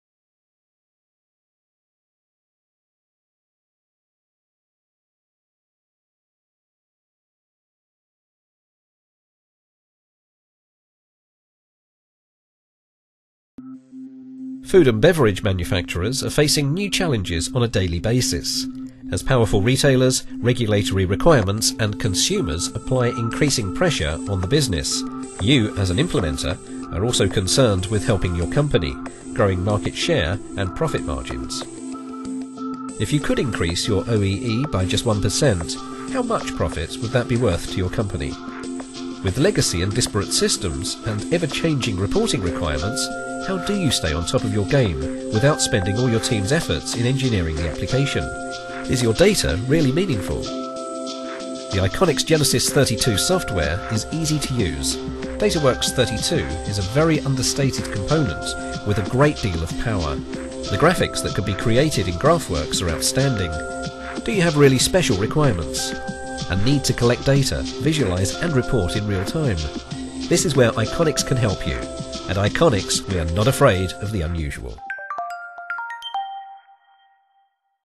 HMI/SCADA Solutions for Food and Beverage Engineers and Operators - ICONICS
ICONICS offers a Food and Beverage HMI/SCADA Solution tailored to IT and Management Roles.  Watch this video to learn more about the technologies behind our visualization and automation solution.